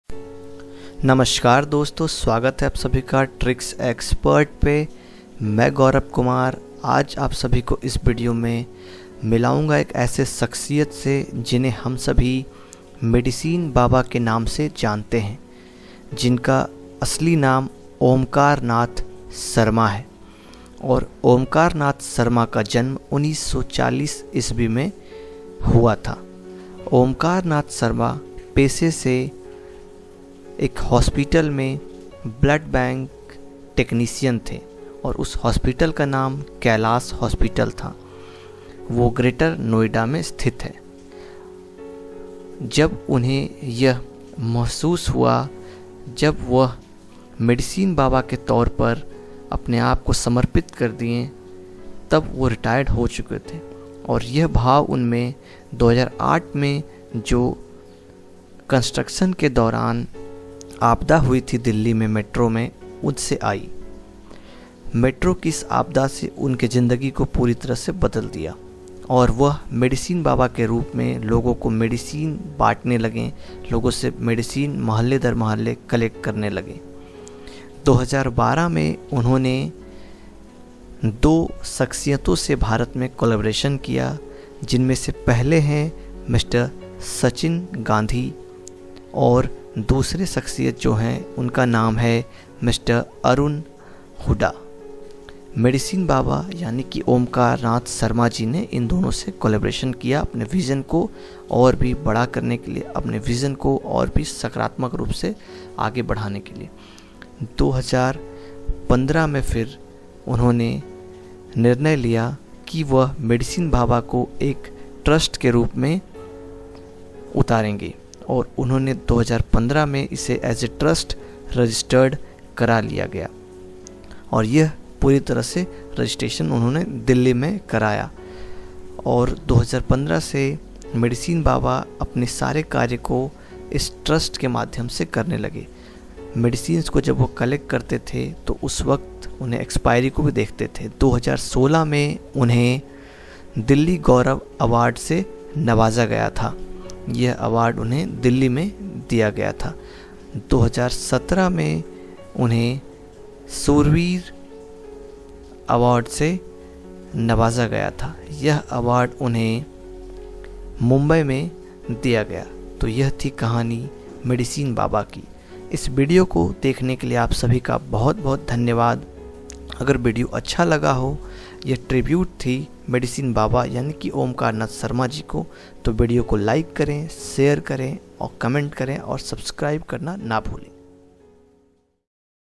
An Inspirational Story And Biography Of Medicine Baba 'Omkar Nath Sharma'
Omkar Nath Ji "Medicine Baba"
born in India (around 1940) also known as the “Medicine Baba” (Medicine Monk). is a retired blood bank technician from a hospital in New Delhi, who voluntarily collects unused medicines from people and distributes them to the poor free of charge.